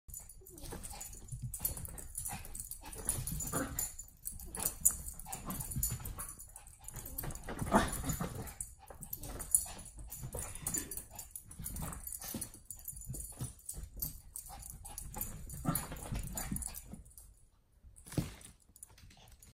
dog meets bunny
this is my dog, Kiwi, meeting my bunny, Bambi for the first time LOL